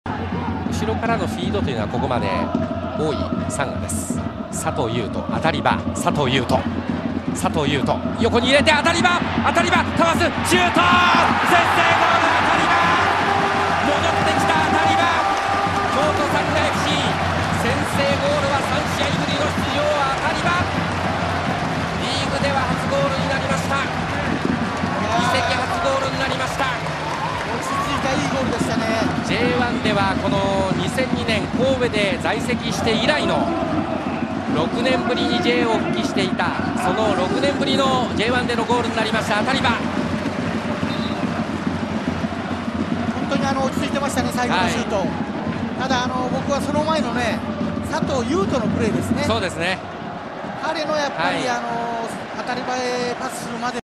Kyoto Sanga FC-Consadole Sapporo 1-0 - Ataliba's Goal
First J-goal for Sanga for the Dark Carioca!! (Dedicated to Paulinho??)